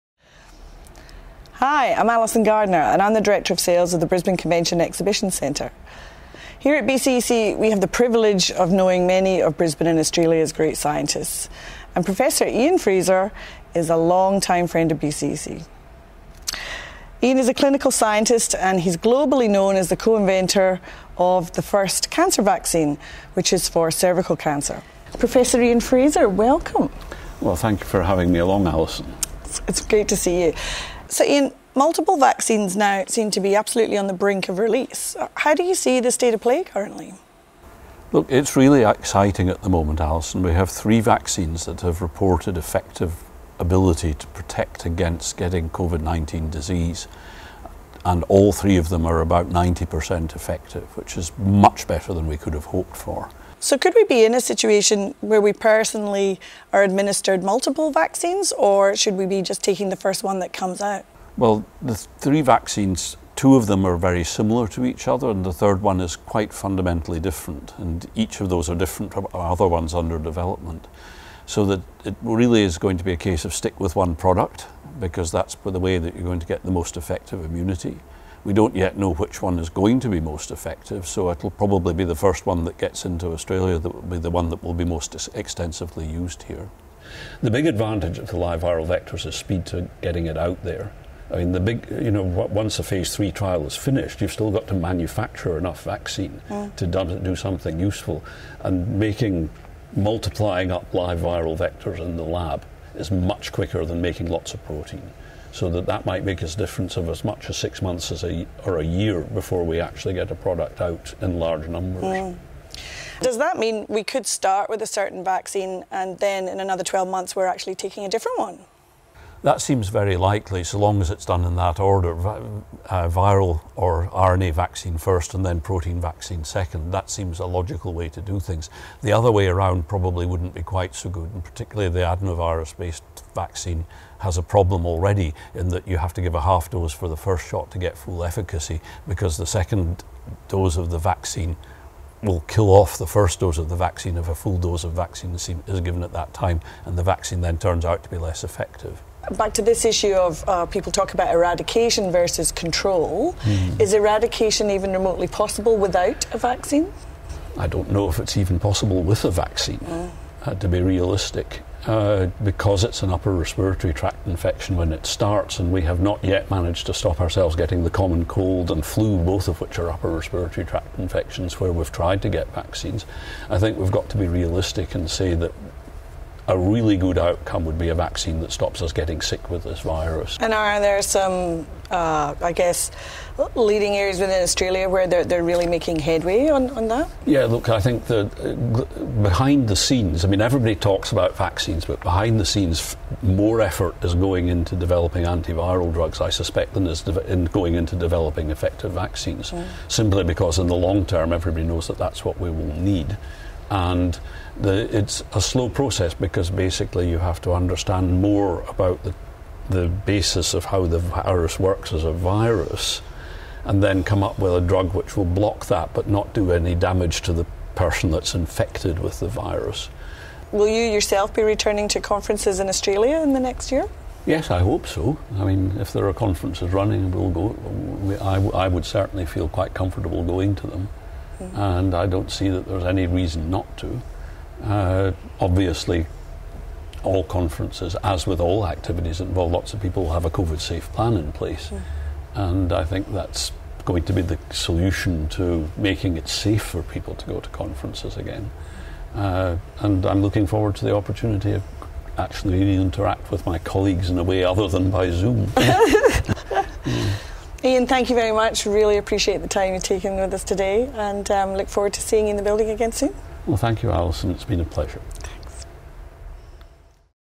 BCEC's Alison Gardiner spends time with world famous immunologist Professor Ian Frazer
As we wait for the first COVID-19 vaccine to be approved and released, we thought you might like to hear from Professor Ian Frazer, an Australian ‘national treasure’ and co-inventor of the world’s first cancer vaccine, Gardasil, currently used world-wide to help prevent cervical cancer.